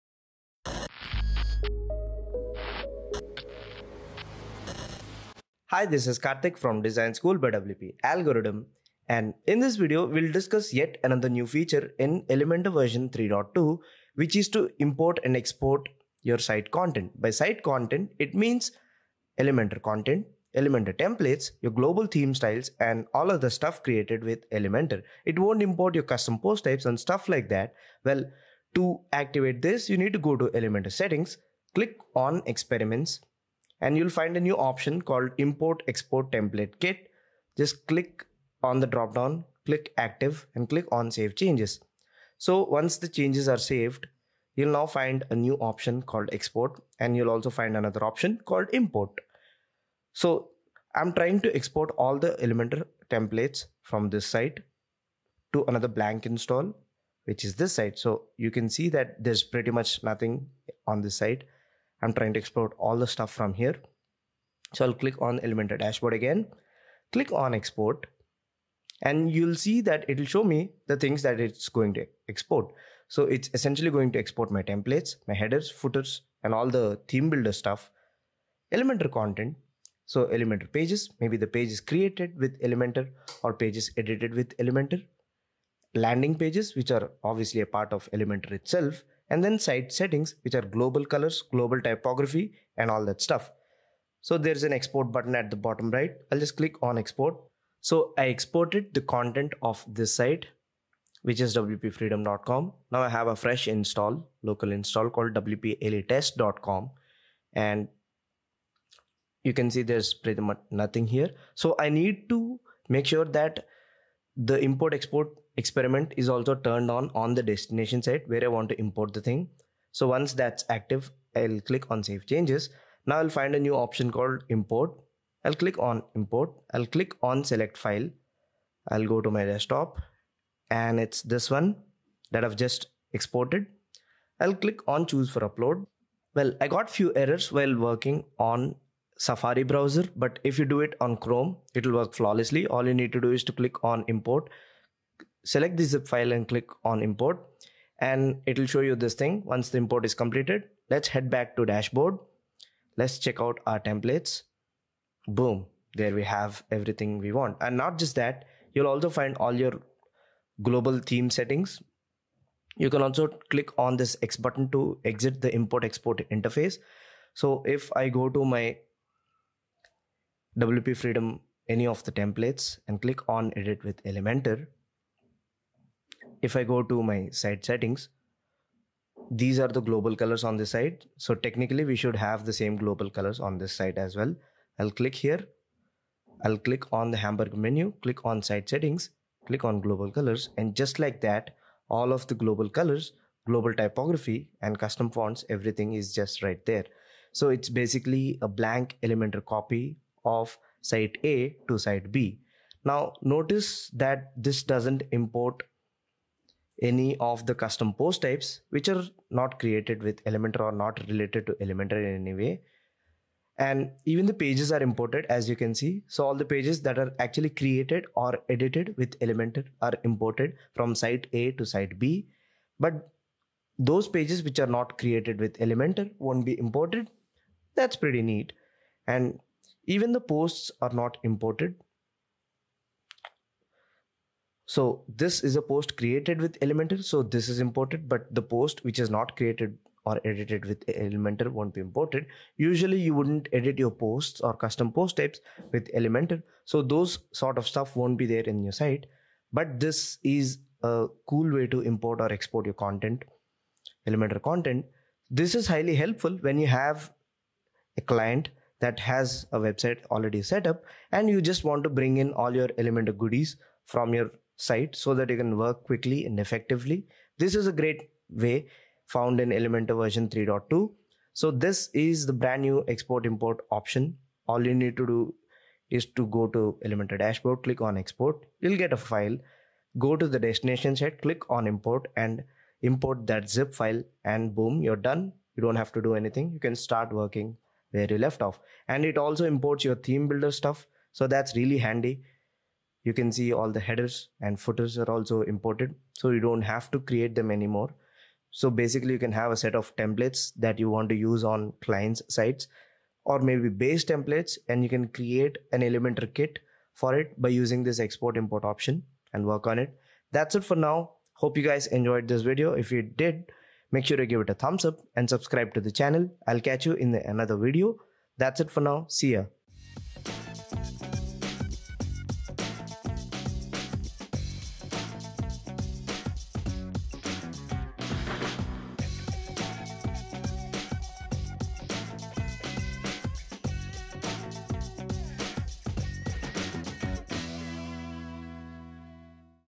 Elementor 3.2 Latest features - Export Import Elementor Site content
Learn how to export/import elementor theme styles, custom fonts, templates and even created pages and posts using new feature in Elementor 3.2.

⚡️⚡️  OUR FINEST RECOMMENDATIONS   ⚡️⚡️⚡️

► ELEMENTOR AND ADD-ONS ◄

► BEST HOSTING ◄